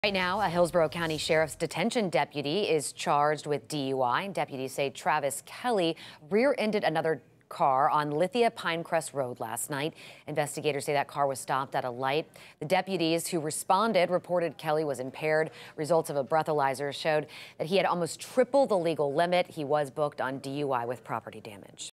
Hillsborough County detention deputy arrested for DUI
A Hillsborough County detention deputy is in jail after being arrested for driving under the influence Thursday evening, according to the sheriff's office.

Around 7:12 p.m., Detention Deputy Travis Kelley was going west on Lithia Pinecrest Road before rear-ending another vehicle that was stopped at a traffic light, deputies wrote in a news release.